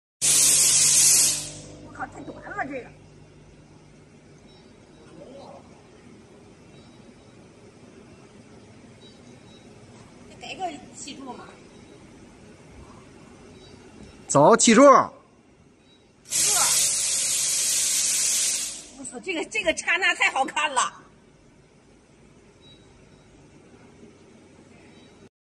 LED Vertical Fog Machine Super-Jet
LED Vertical Fog Machine Super-Jet, it is specially designed for professional stage, with the unique "super-jet" technology, the output speed and strength can even be comparable with CO2 jet.